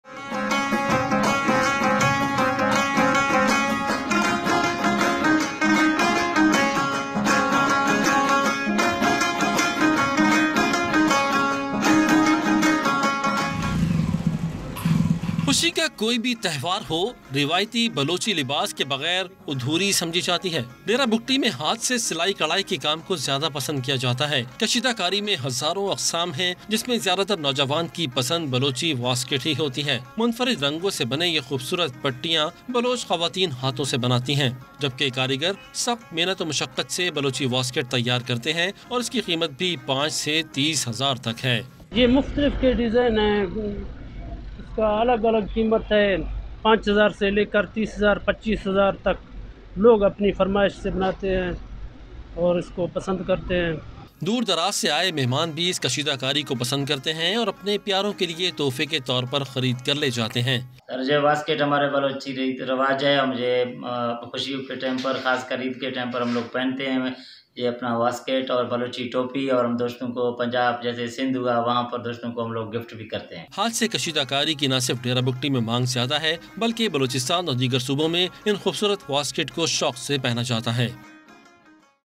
EID BALOCHI CULTURAL DERA BIGTHI PKG 26 07
Report on Traditional Balochi West coats of Dera Bugti.
balochi culture female dress
balochi dress for male
balochi dress 2020